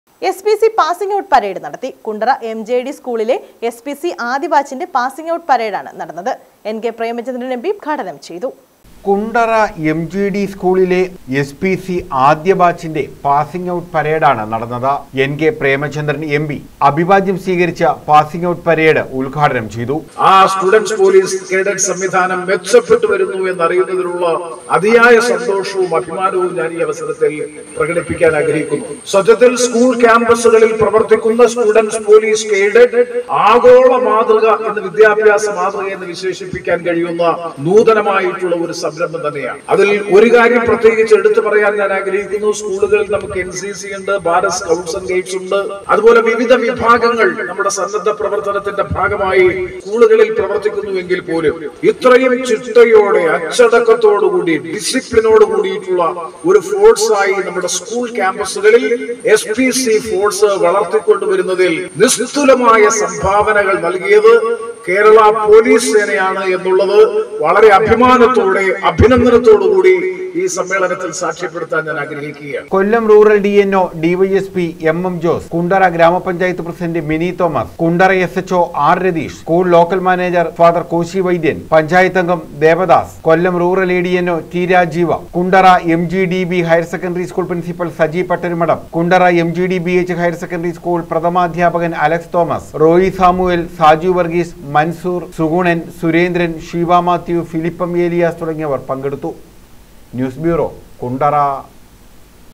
എസ്.പി.സി പാസിങ് ഔട്ട് പരേഡ് നടത്തി
എസ്.പി.സി പാസിങ് ഔട്ട് പരേഡ് നടത്തി. കുണ്ടറ എം.ജി.ഡി സ്‌കൂളിലെ എസ്.പി.സി ആദ്യ ബാച്ചിന്റെ പാസിങ്ഔട്ട് പരേഡാണ് നടന്നത്. എന്‍.കെ പ്രേമചന്ദ്രന്‍ എം.പി പരേഡ് സ്വീകരിച്ച് ഉദ്ഘാടനം ചെയ്തു.